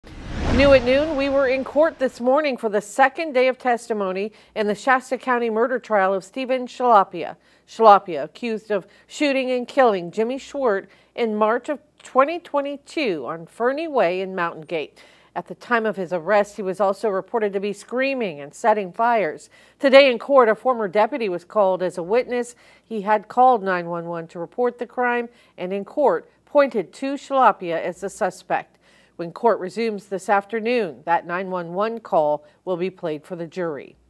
Accused murderer makes appearance in Shasta County court
Day two of testimony has begun in the murder trial of Steven Schlapia.